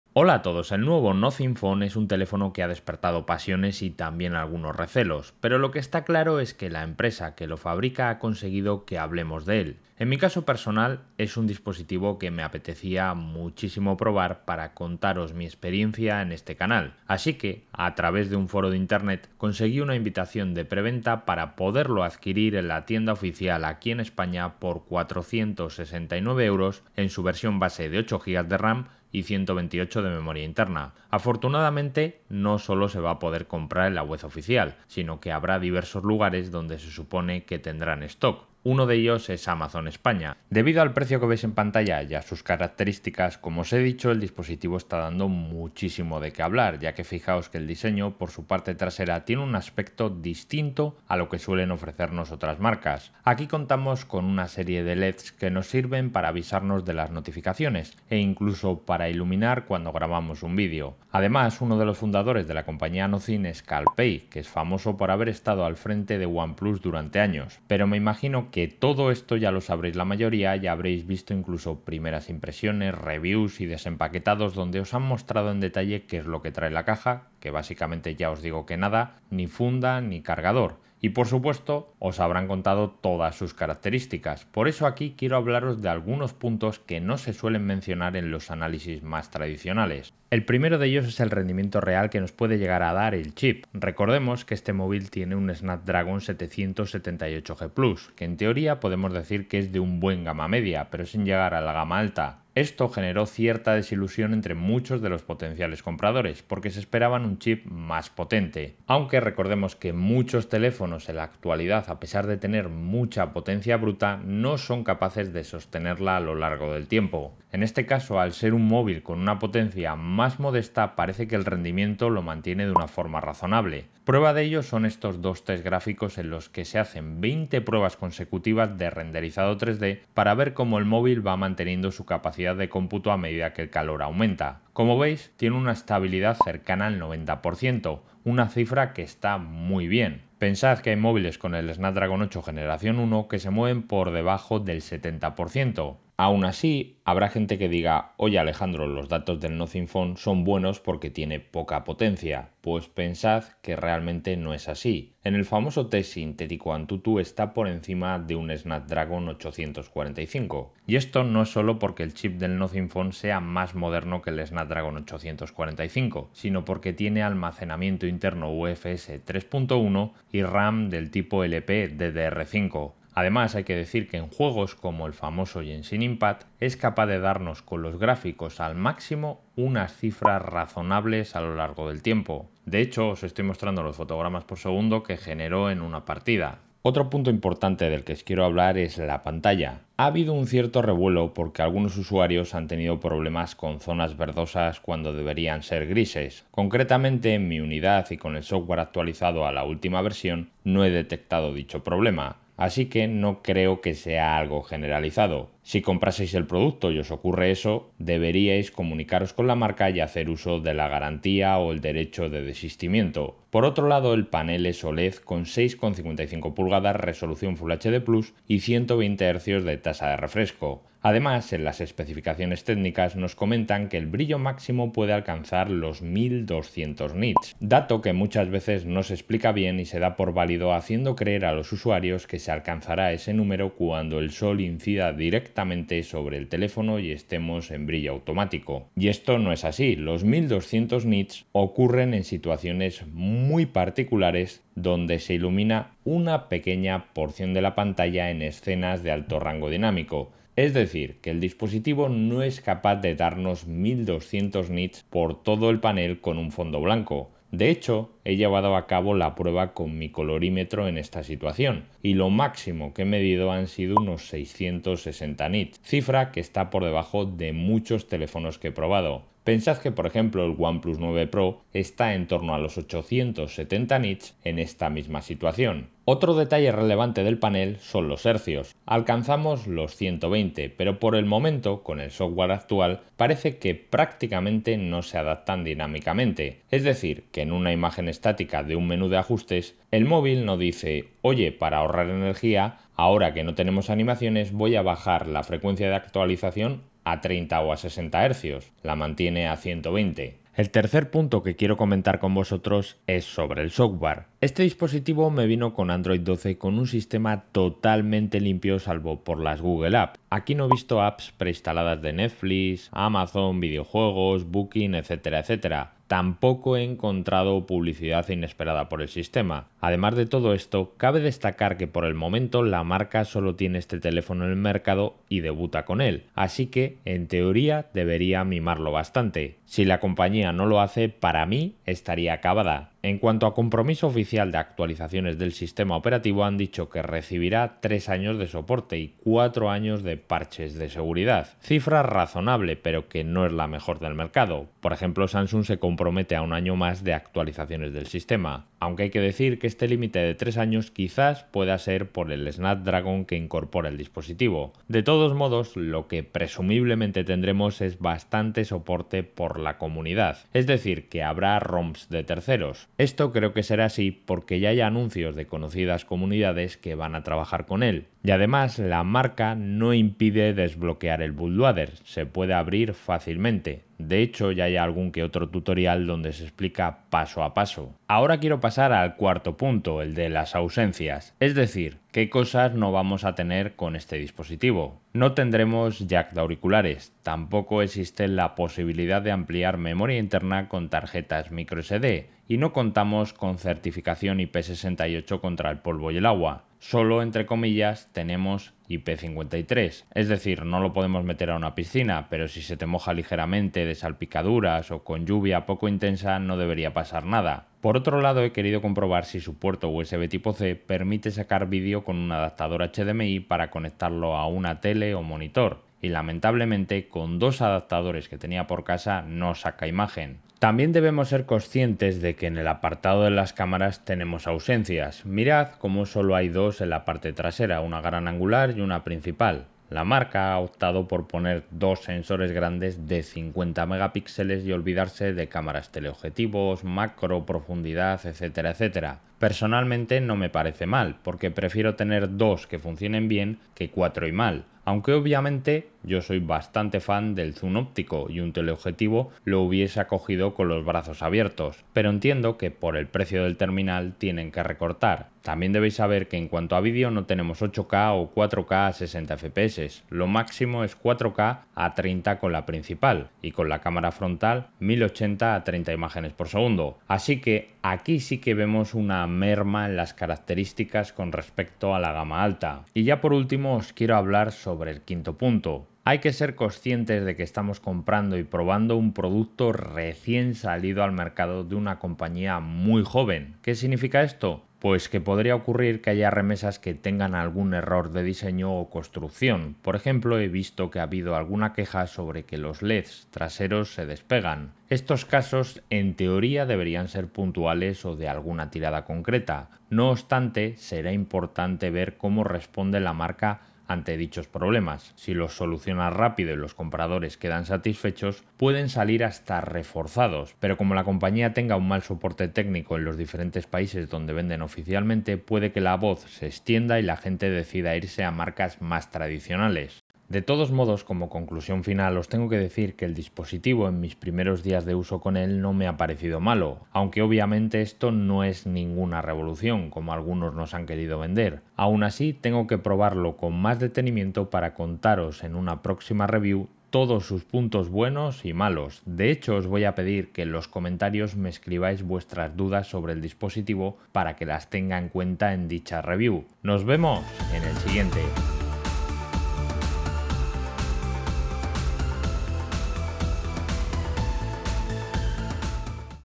Nothing Phone 1 | 5 Cosas Que Quizá NADIE TE CONTÓ
Os hablo de algunos puntos sobre el nuevo Nothing Phone 1 que se han comentado poco o directamente no se han mencionado hasta ahora.

Nothing Phone 1 en Amazon España

Telegram con chollos y ofertas diarias de productos analizados en este canal de Youtube